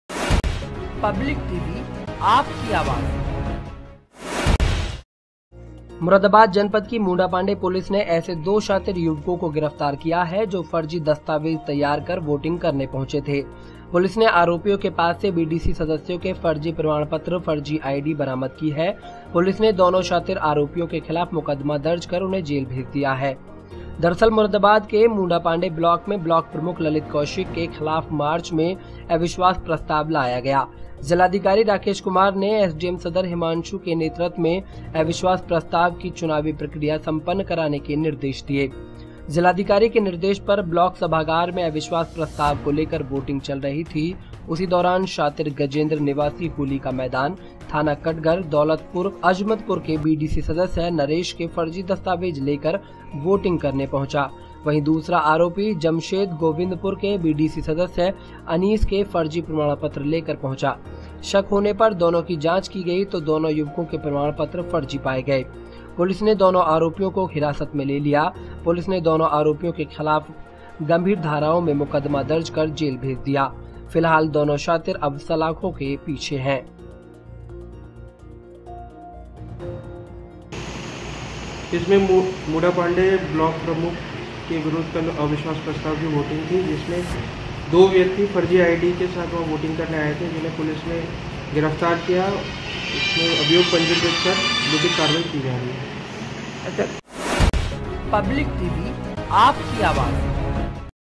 ब्लॉक प्रमुखी के अविश्वास प्रताव को लेकर चुनाव में फर्जी वोटिंग करते पुलिस ने दो पकड़े
Election#Moradabad#Police